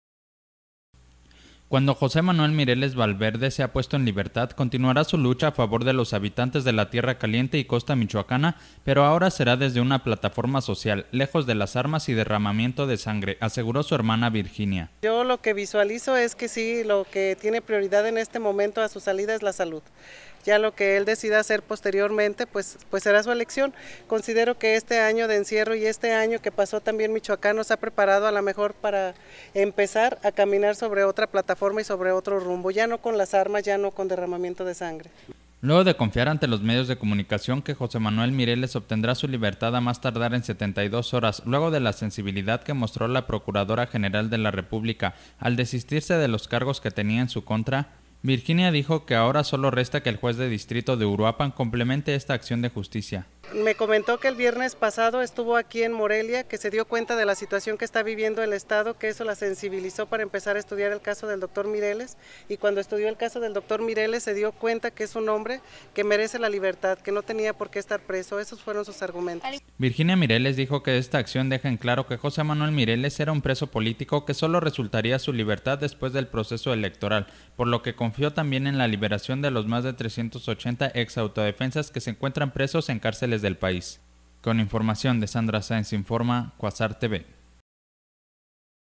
Mireles dejará las armas cuando salga de la cárcel; Virginia
Cuando José Manuel Mireles Valverde tenga su libertad continuará su lucha a favor de los habitantes de la tierra caliente y costa michoacana pero ahora será desde una plataforma social, lejos de las armas y derramamiento de sangre, aseguró su hermana Virginia...